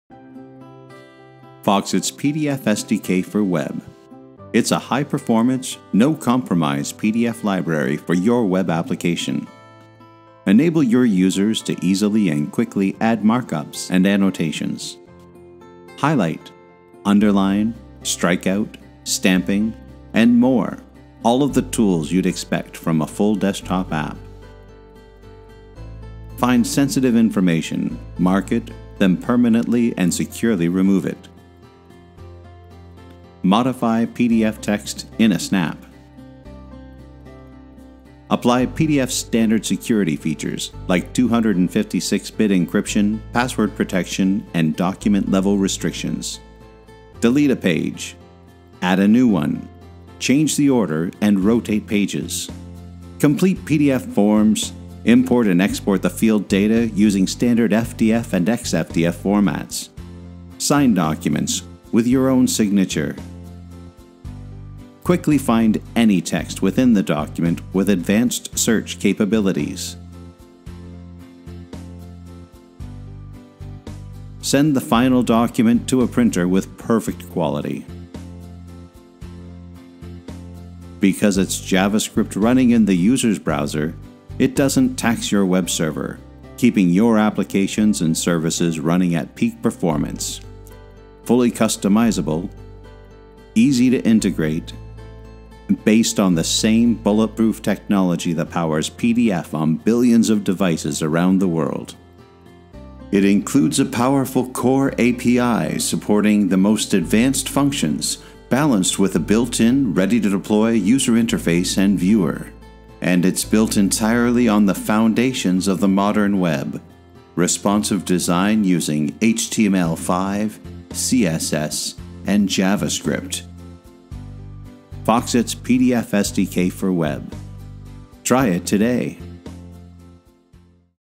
Foxit PDF SDK for Web
Open, view and annotate PDF files on both mobile and desktop devices, in a full PDF viewer and editor in your browser using JavaScript. As a purely JavaScript library, our product requires no plug-ins and provides native-like performance no matter where you use it.